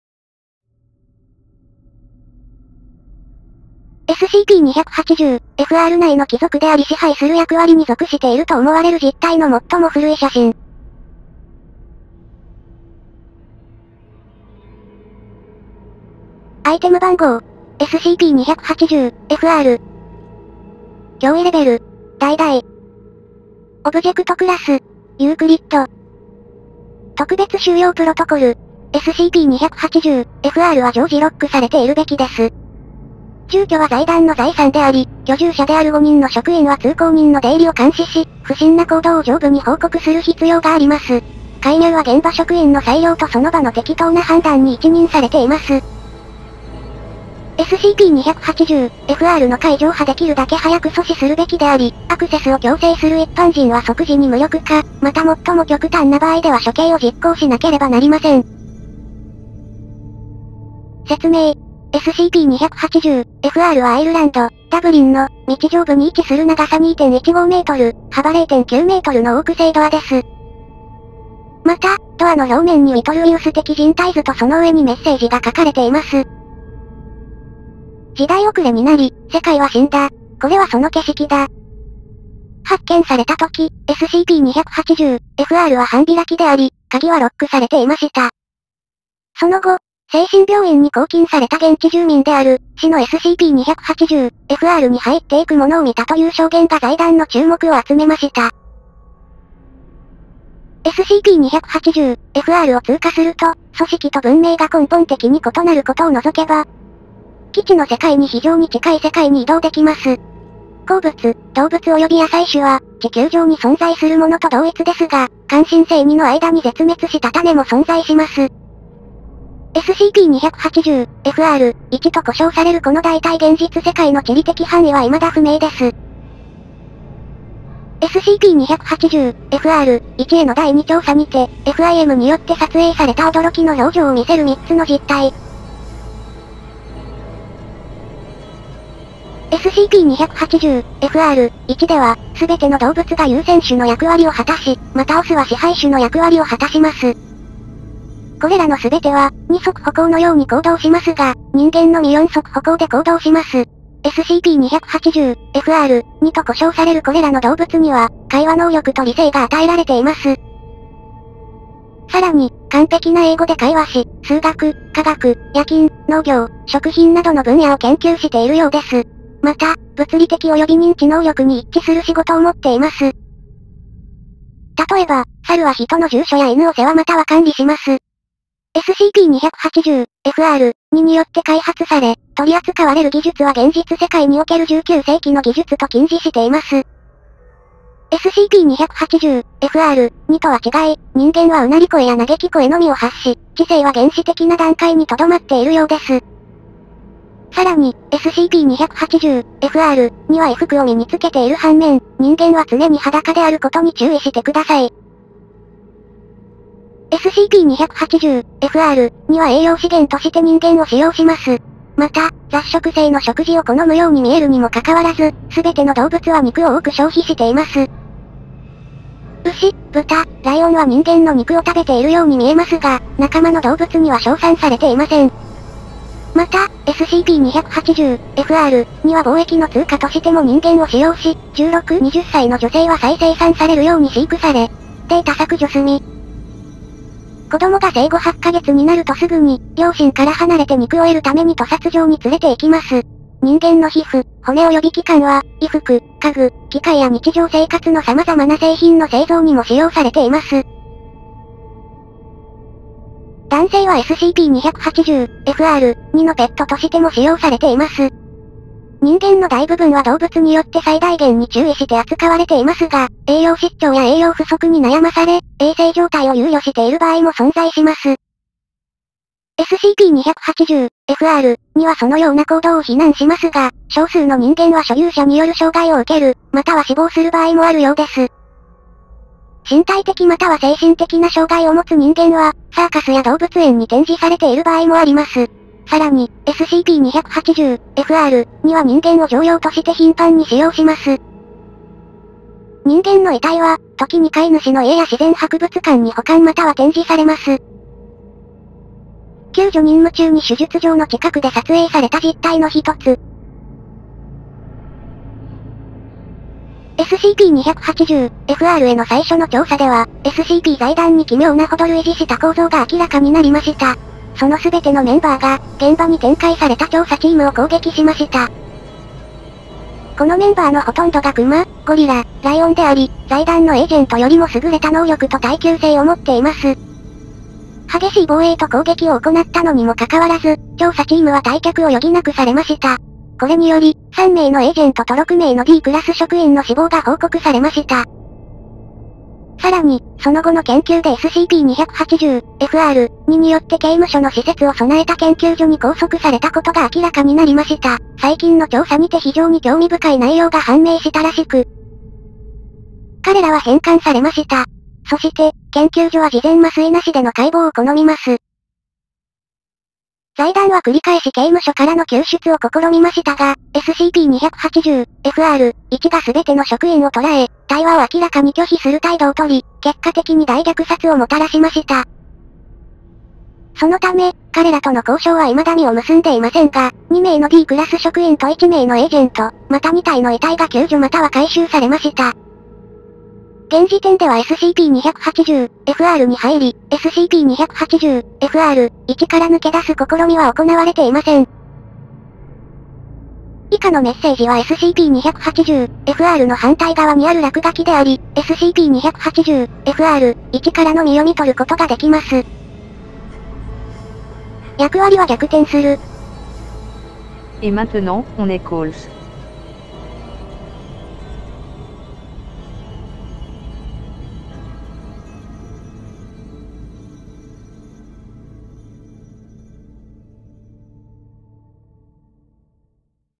SCP-280-FR - 明くる日の動物たち
【寸評】怖い度 ★★★☆☆：異世界進入。人と動物の立場逆転
アイテム番号: SCP-280-FR
脅威レベル: 橙 ●
オブジェクトクラス: Euclid

《HachiStudio》 シナリオ読込型VRoidビューワ HachiViewerPlus